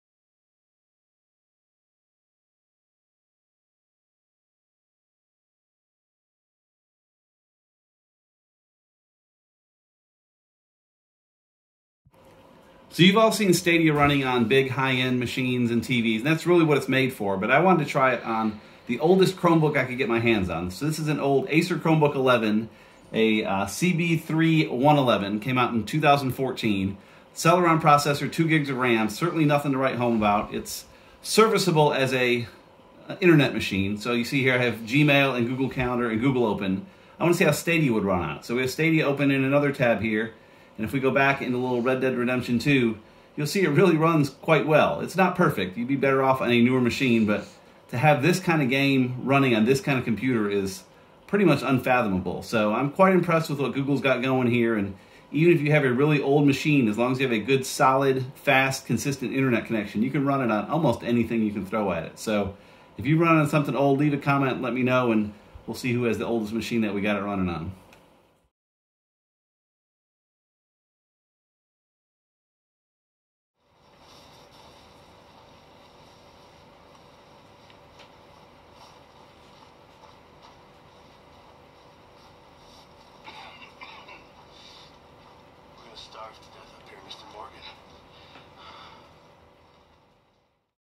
Running Google Stadia on an old Chromebook
The promise of Stadia is amazing; as long as you have a solid internet connection, you can run it on almost any device.

But what happens if you try it on a five-year old, pitiful Chromebook? That's what I just did, and the results were surprisingly good. I ran it on an Acer Chromebook 11 (CB3-111-C8UB) -- it came out in 2014, has 2GB RAM and runs on a Celeron chip.